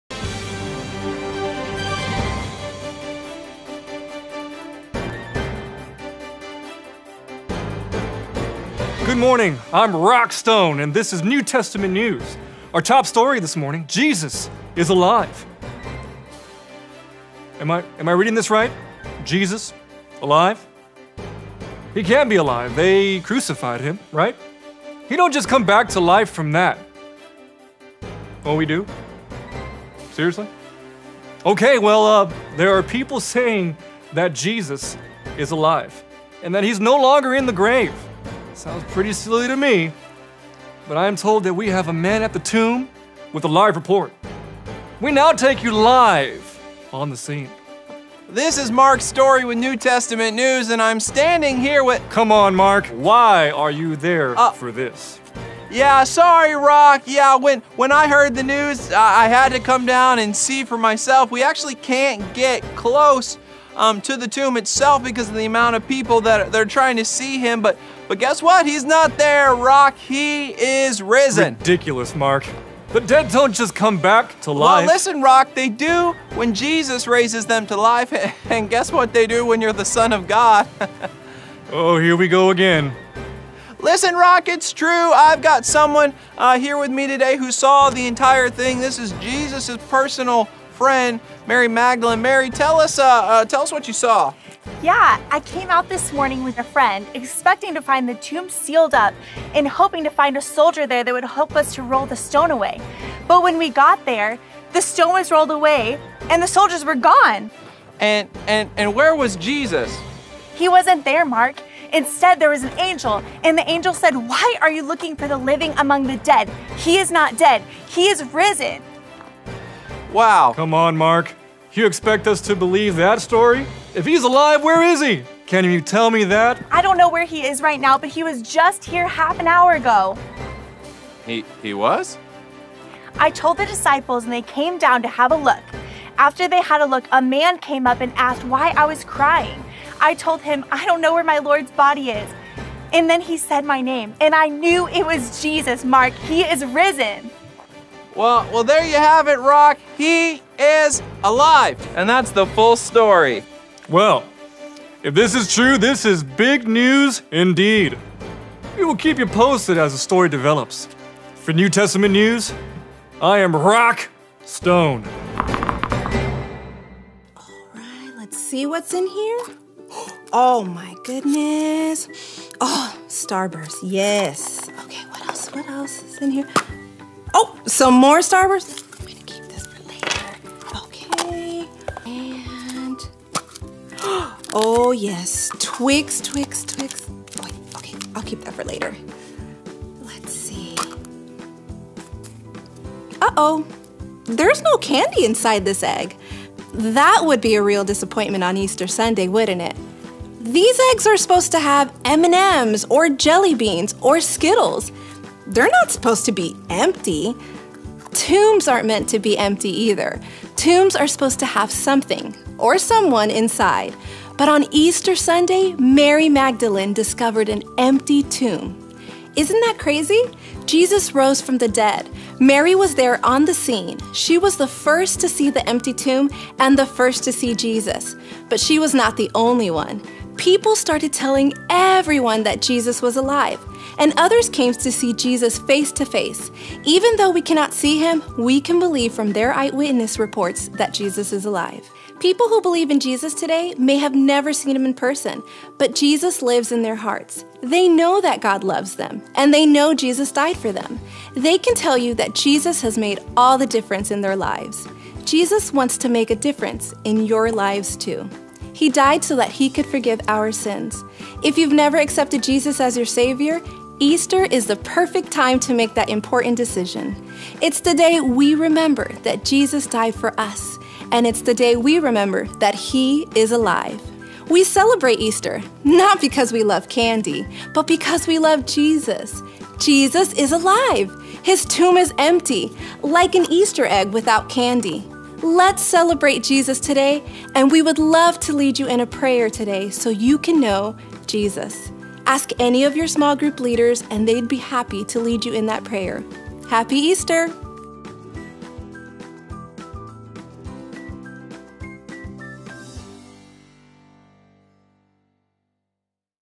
The Empty Tomb | ROCK STONE and MARK STORY | Easter Lesson
Check out this fun way to tell kids about how Jesus is alive! He is not in the tomb and He has risen. Watch to see watch Mark Story finds out "ON SCENE" as he reports with New Testament News with anchor Rock Stone. Stick around for a fun way to remind your kids about the tomb being empty on Easter after the report!